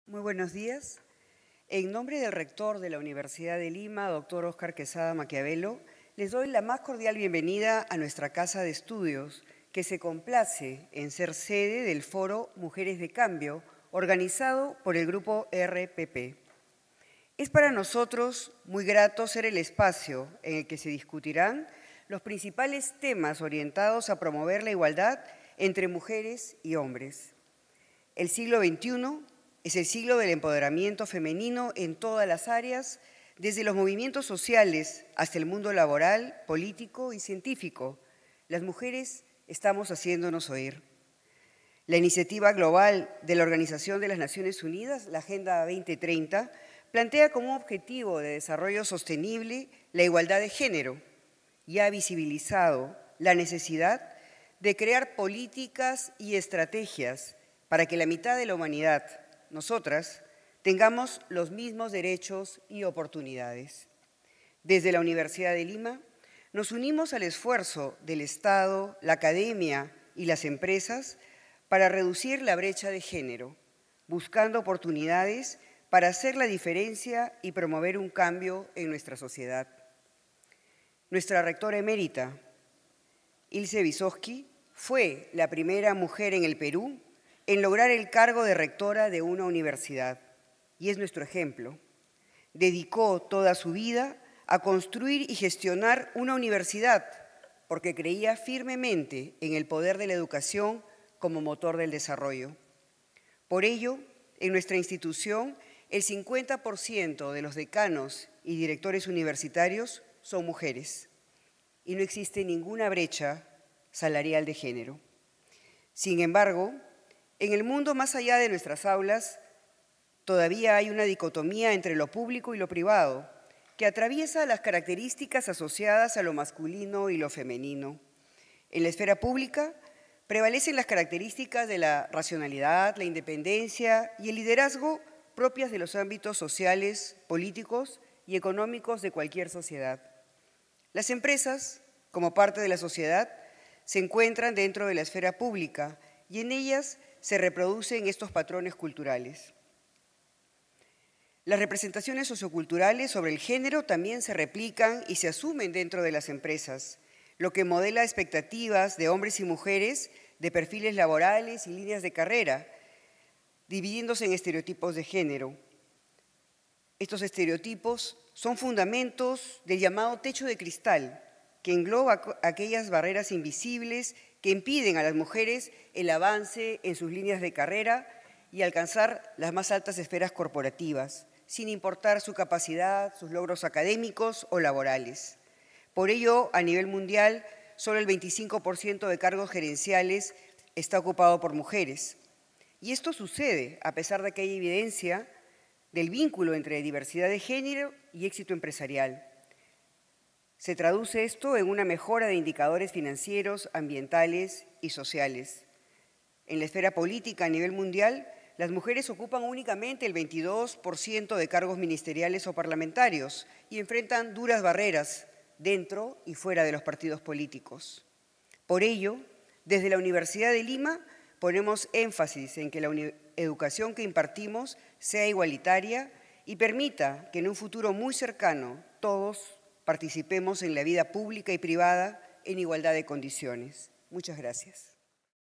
Palabras de bienvenida Patricia Stuart Alvarado decana de la ULIMA| Mujeres de cambio RPP Radio
Patricia Stuart Alvarado, decana de la Facultad de Ciencias Empresariales y Económicas, habló sobre importancia de la igualdad de género en el país.descripción predeterminado